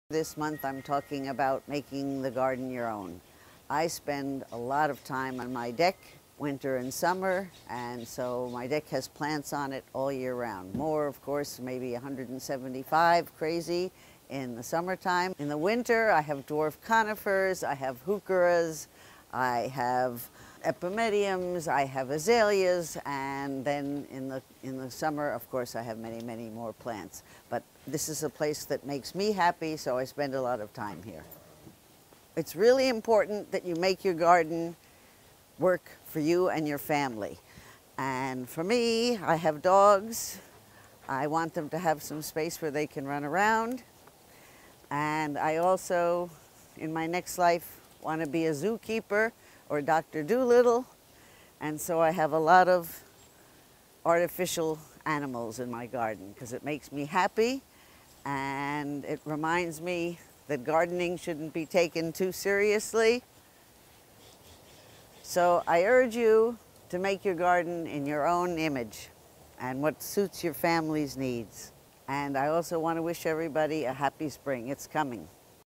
Making the garden your own
Ellen Schlesinger on making a garden your own with potted plants, art, natural elements like rocks and driftwood. (Collin Andrew/The Register-Guard)